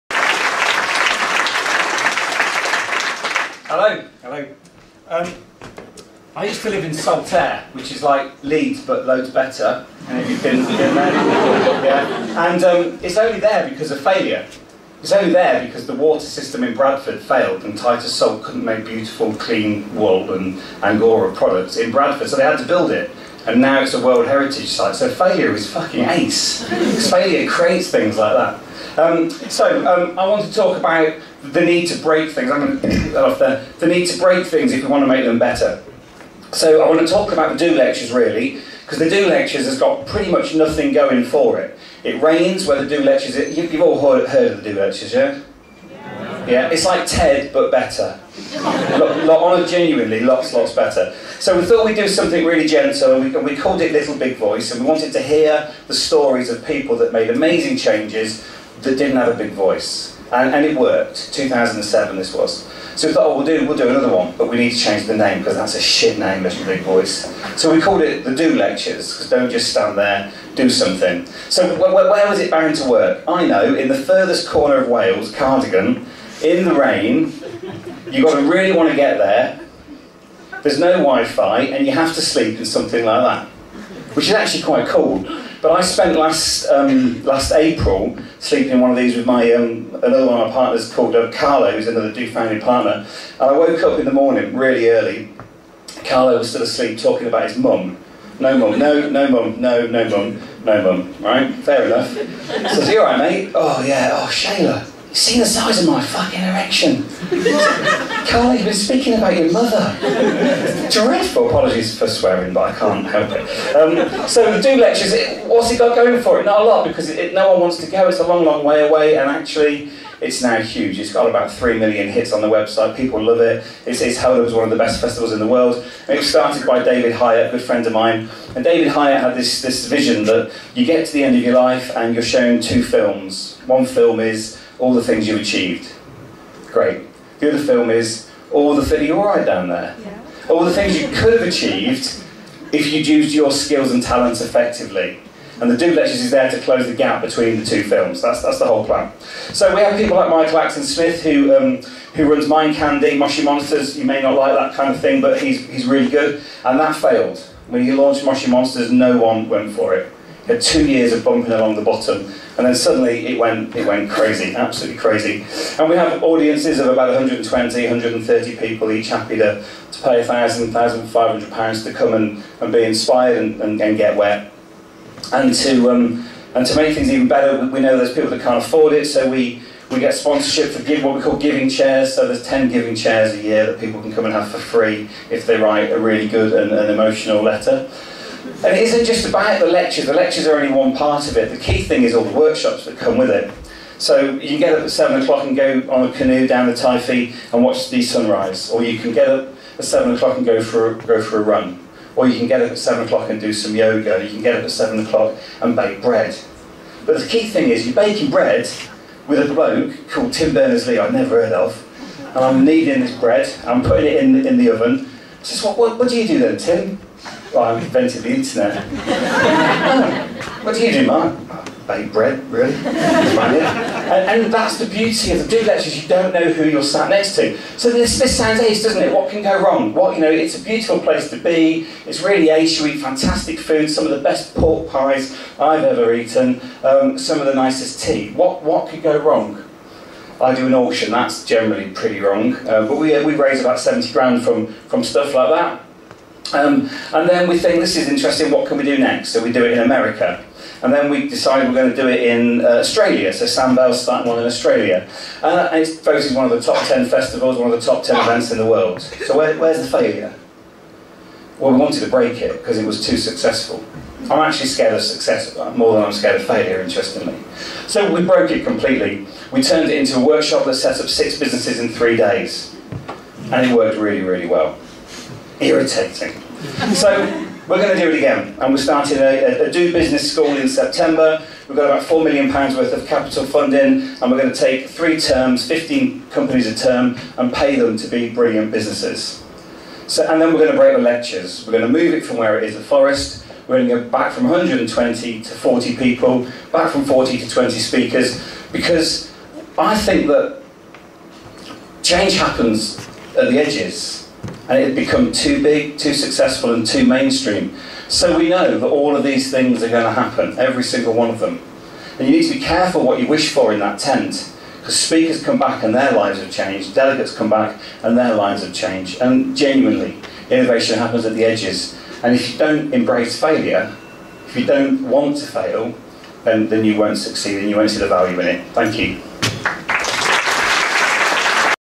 Mark Shayler at Bettakultcha - The Importance of Failure
Mark Shayer at Bettakultcha, The Importance of Failure